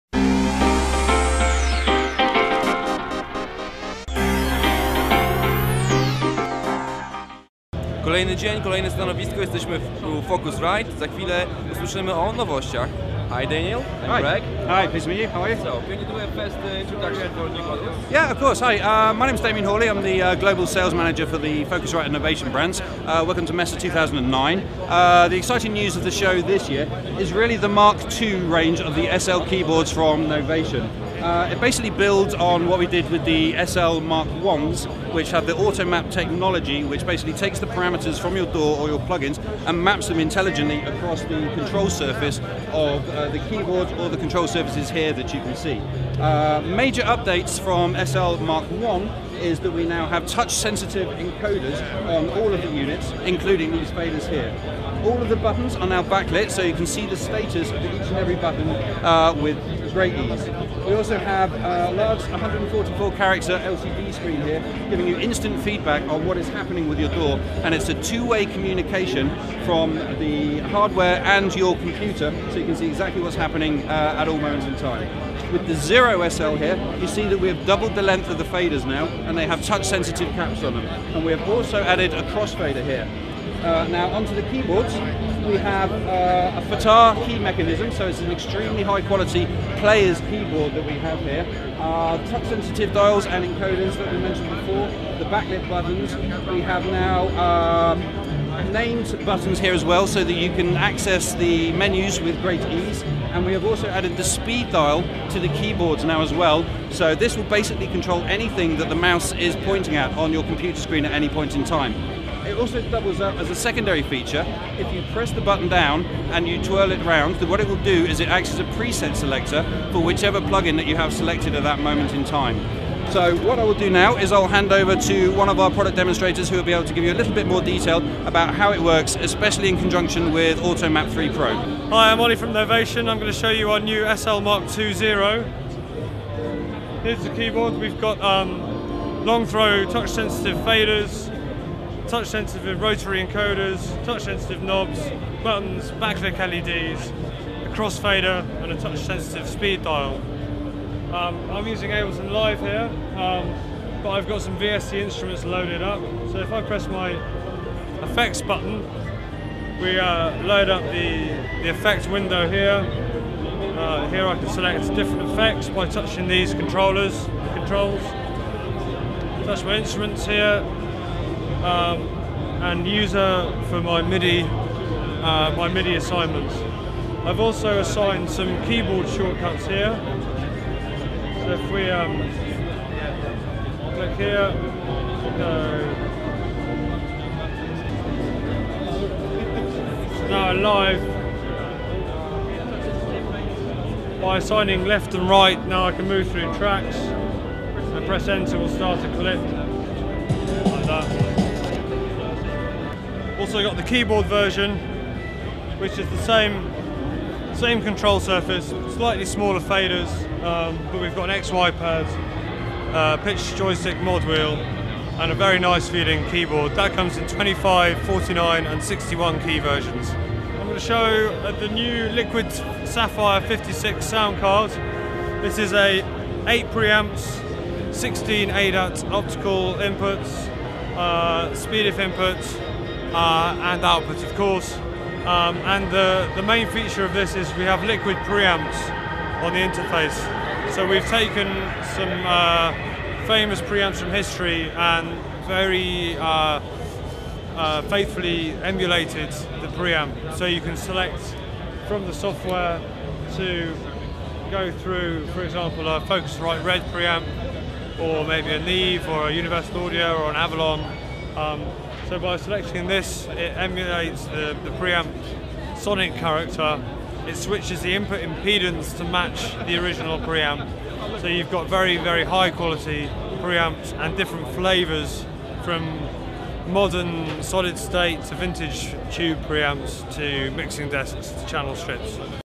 Focusrite (Musik Messe 2009)
video relacja INFOMUSIC.PL - z targów Musik Messe 2009